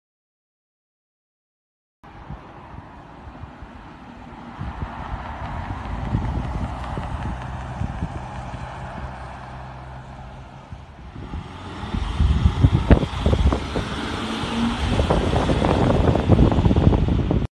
Steuben County AMR ALS 101 & Bath Ambulance B1 Responding
Bath Ambulance B1 and AMR ALS101 were requested by Cameron Ambulance for a 1 car mva with 2 injured people on County Route 10 in the Town of Cameron. B1 transported 1 to IRA and so did Cameron Ambulance. No Fire Department was needed on scene and the scene cleared fairly quickly. B1 did go no lights no sirens and 101 went no sirens with lights due to the scene already under control and the scene itself being right up over the hill form where I was recording.